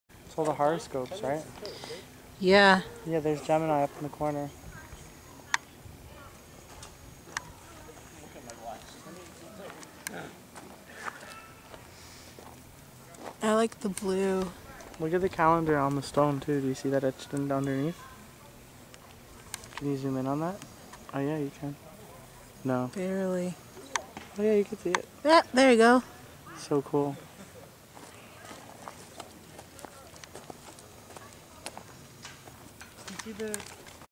Bok Tower 3
Jay and I went on vacation to Florida and spent 8 days with his folks. Here, they took us to Bok Tower. Absolutely stunning.